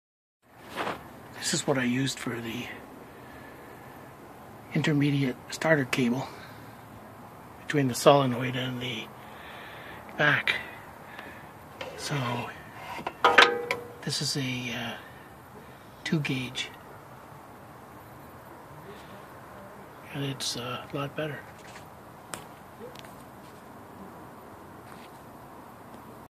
Chevelle Rear Battery Relocate - 2 Gauge Extension
This 1968 Chevelle SS396 runs a rear battery setup with a 2 gaugewelding cable positive to the front installed in the eighties and then an extension to a Ford solenoid on the fender. Replaced the thin 4 gauge extension with a custom made 2 gauge cable with the corroded portion cut off and a new copper lug soldered on. The GM starter solenoid has the crank connection joined to the large positive cable from the Ford solenoid. Big improvement in starting.